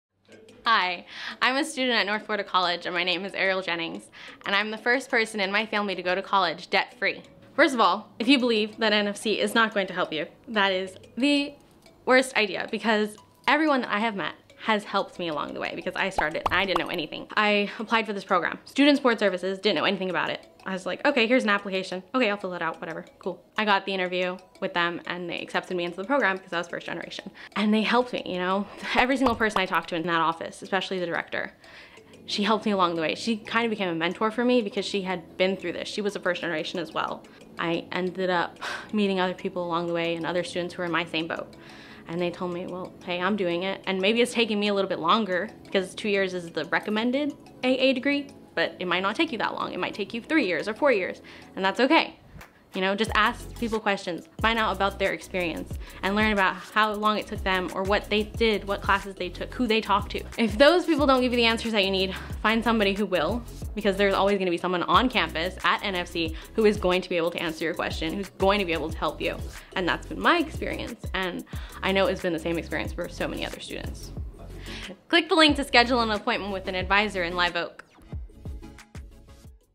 Watch Ariel Jenning's Story About North Florida College - Live Oak Location
Watch Ariel Jenning's story about how North Florida College lets you save money and focus on what matters - getting an education without being crushed by debt.  Talk to an Advisor in Live Oak today.